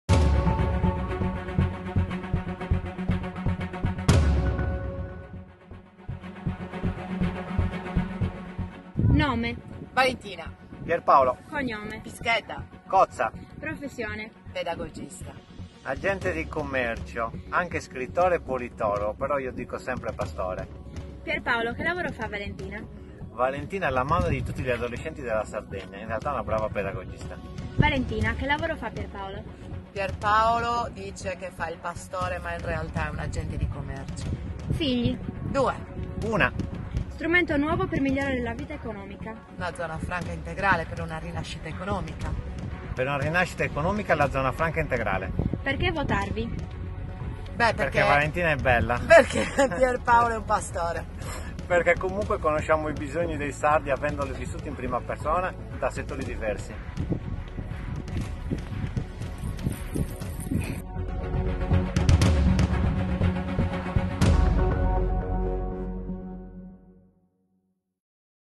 Conosciamo i nostri candidati con l’intervista di una piccola ienetta
Elezioni regionali Sardegna 24 febbraio 2019